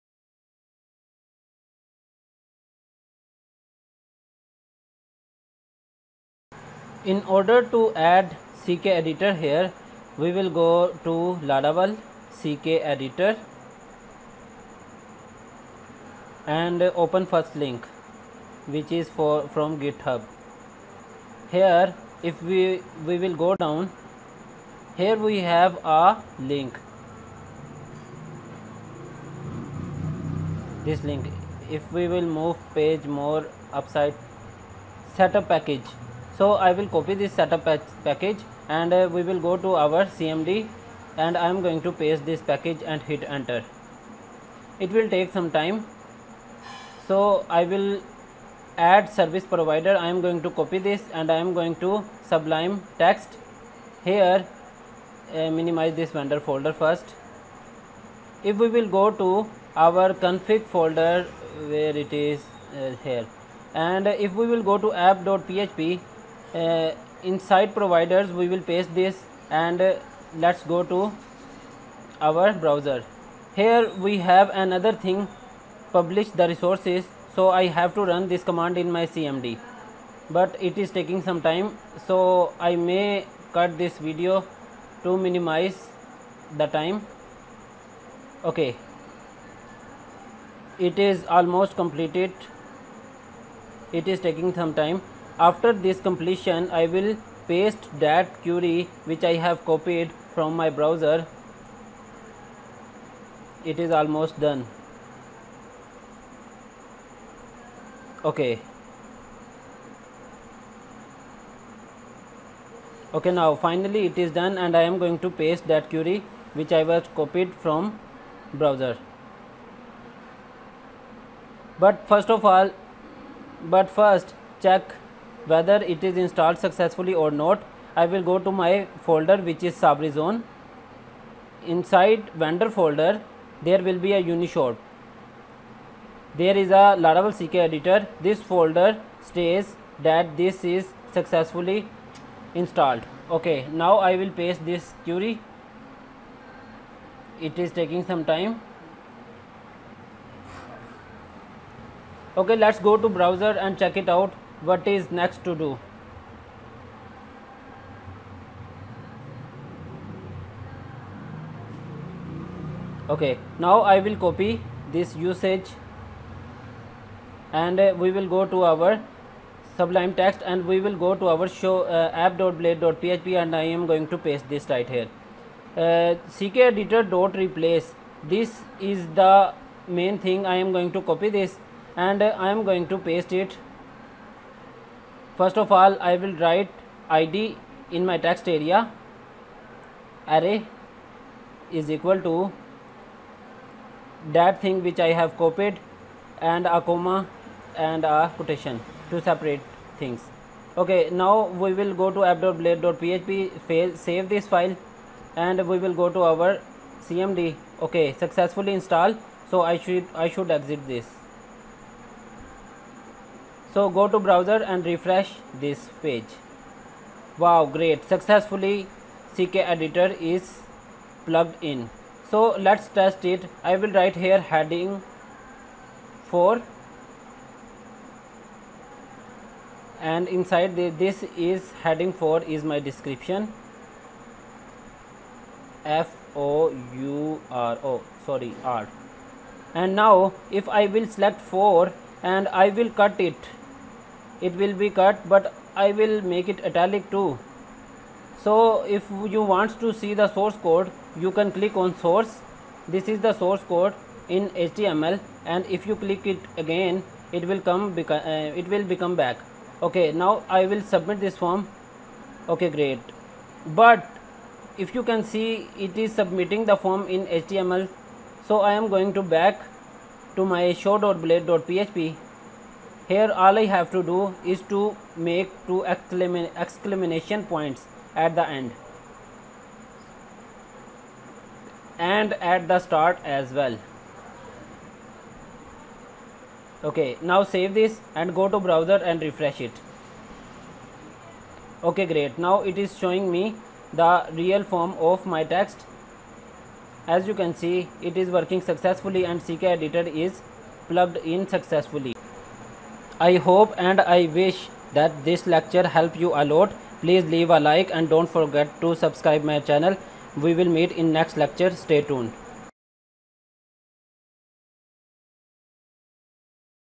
Laravel Ckeditor | Installation and proper use of ck editor
In this Lecture i will teach you how you can add ckeditor to your laravel project
i hope this video will help you a lots
you can also learn laravel from Scratch From my YouTube Chanel

for more Please
ii)Like
iii)Comment
iv)Share
if you wants to help